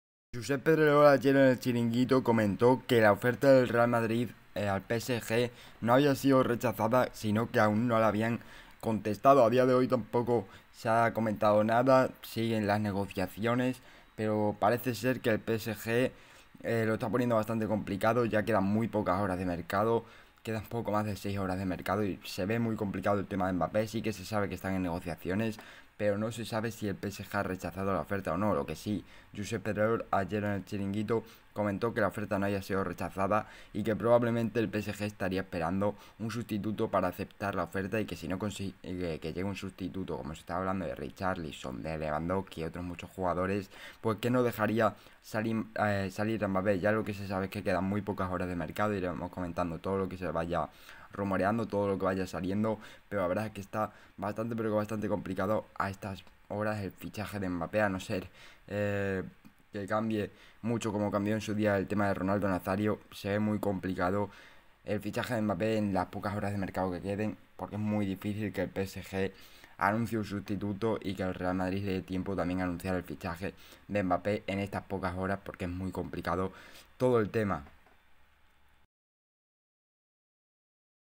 Josep Pedrerol suelta la BOMBA en El Chiringuito y confirma que el PSG no contesta al Madrid
✅ En este vídeo comentaremos todo sobre la ULTIMA INFORMACION de JOSEP PEDREROL y CONFIRMA que las NEGOCIACIONES NO ESTAN ROTAS entre el REAL MADRID y el PSG.